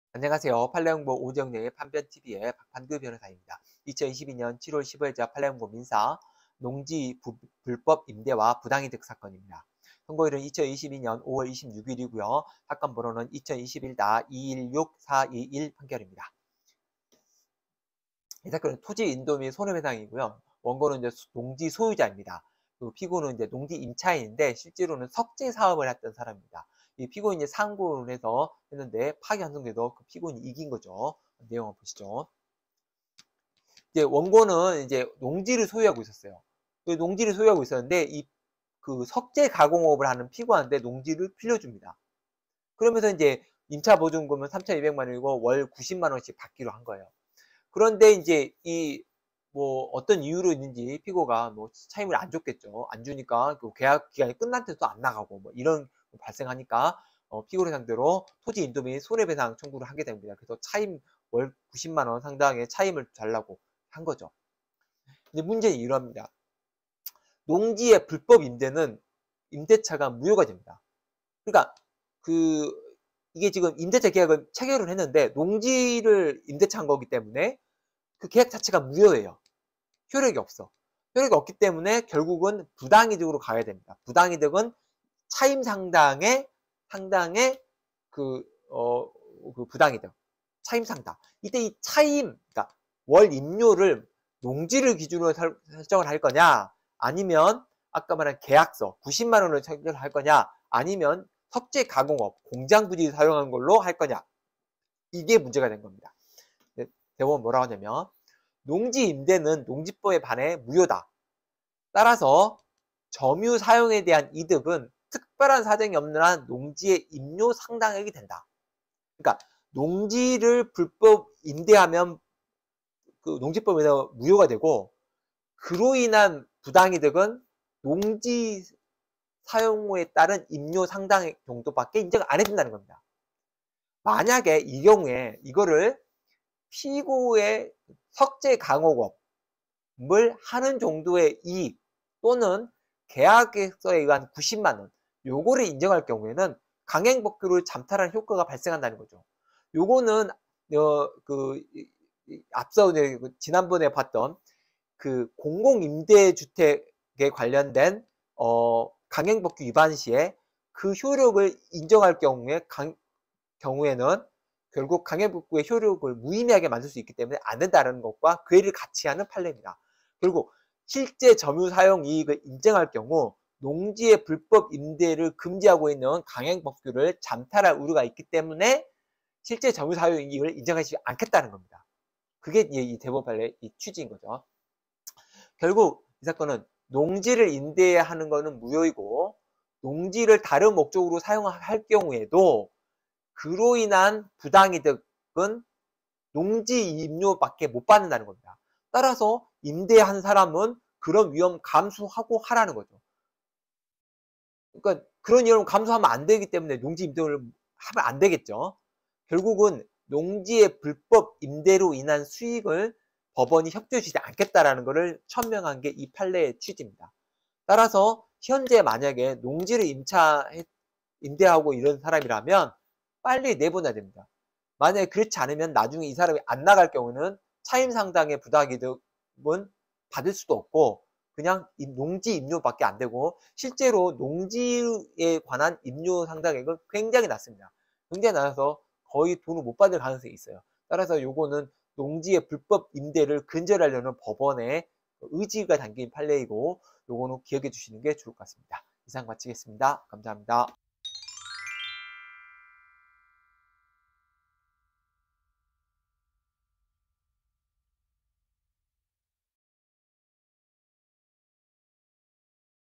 농지 불법임대와 부당이득 사건 [22.7.15.자 판례공보(민사)]
농지 불법임대와 부당이득 사건
별 4개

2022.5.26. 선고 2021다216421 등 판결
토지인도등, 손해배상

원고 농지소유자
피고 농지임차인(상고인)

파기환송

원고는 농지를 소유하고 있었음
원고는 석재가공업을 하는 피고에게 농지를 임대함
임차보증금 3,200만원, 월 90만원
농지 이용시에는 월 차임이 현저히 적음

농지의 불법 임대시 임대차 무효임
부당이득 반환시 임료는 사용이익인지, 농지이용이익인지

농지임대는 농지법에 위반하여 무효
점유사용에 따른 이득은 특별한 사정이 없는 한 농지의 임료 상당액임
실제 점유사용이익을 인정할 경우 강행법규 잠탈 효과 발생함

실무활용
농지 임대는 금지
농지를 다른 목적으로 사용하게 할 경우
부당이득 청구는 농지의 임료 상당액
농지 불법임대로 인한 수익을 법원이 협조할 수 없음